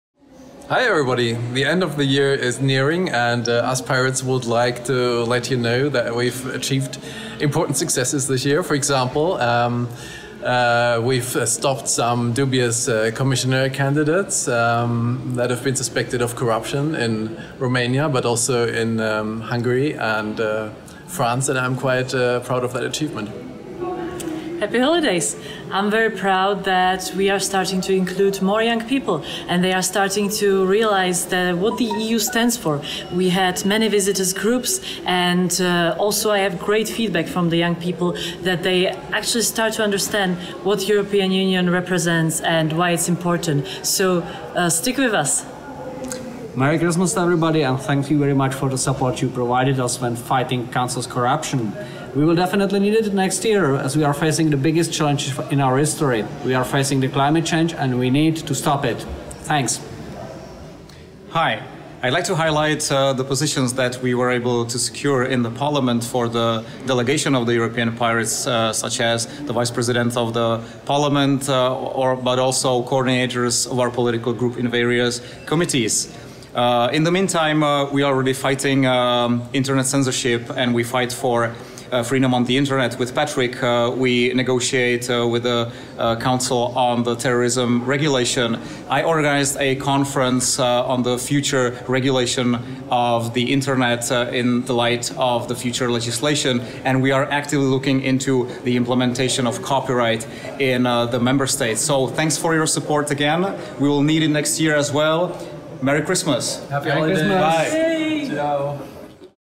Unsere ersten Monate als Europaabgeordnete der europäischen Piratenpartei | Jahresrückblick 2019
In diesem Video geben die vier Europaabgeordneten der europäischen Piratenparteien, Patrick Breyer, Markéta Gregorová, Mikuláš Peksa und Marcel Kolaja, einen kurzen Jahresrückblick auf das Jahr 2019 und ihr ersten Monate im Europaparlament. Dabei berichten sie über ihre ersten wichtigen Erfolge als Europaabgeordnete, wie z.B. das Stoppen einiger zweifelhafter KommissionskandidatInnen, die Sicherung einiger wichtiger Positionen und das Motivieren vieler junger Leute für (Europa-)Politik. (deutscher Untertitel verfügbar)

Danke für all Euren Support und schöne Feiertage! :)

Mehr Informationen über die PIRATEN findest Du hier: @ Werde jetzt Mitglied unter: @irat/